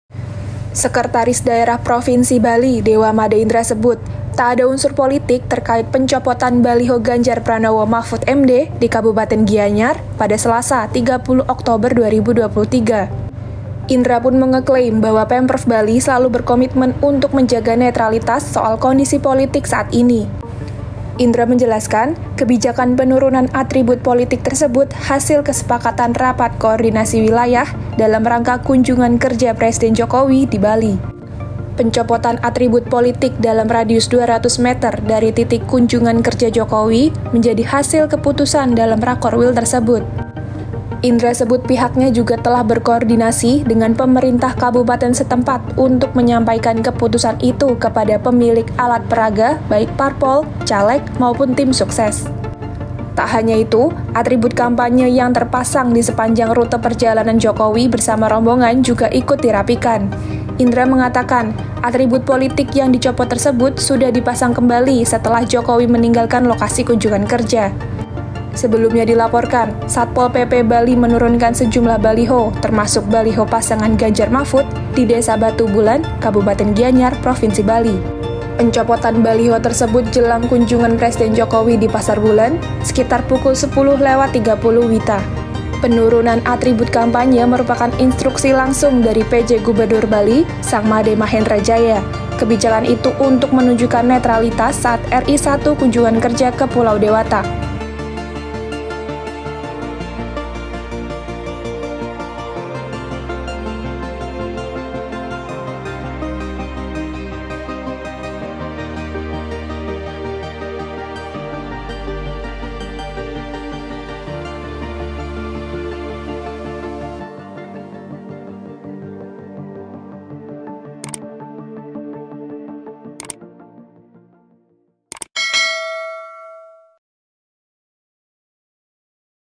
Penjelasan Pemprov Bali soal Pencopotan Baliho Ganjar-Mahfud Saat Kunker Jokowi
Sekretaris Daerah (Sekda) Provinsi Bali, Dewa Made Indra, menegaskan tidak ada unsur politik dalam pencopotan sejumlah baliho termasuk baliho Ganjar-Mahfud MD di Desa Batu Bulan, Kabupaten Gianyar, Bali, pada Selasa (30/10/2023).

Ia menjelaskan, kebijakan penurunan atribut politik tersebut merupakan hasil kesepakatan Rapat Koordinasi Wilayah (Rakorwil) dalam rangka kunjungan kerja Presiden Joko Widodo di Pulau Dewata. Dalam rapat tersebut diputuskan bahwa segala atribut partai politik dalam radius 200 meter dari titik kunjungan kerja Kepala Negara dibersihkan. Selain itu, pihaknya juga telah berkoordinasi dengan pemerintah kabupaten setempat untuk menyampaikan keputusan tersebut kepada pemilik alat peraga baik partai politik, calon legislatif, maupun tim sukses.

Penulis: Yohanes Valdi Seriang Ginta
Penulis Naskah: Vina Muthi Ambarwati
Narator: Vina Muthi Ambarwati
Video Editor: Menika Ambar Sari
Produser: Firzha Ananda Putri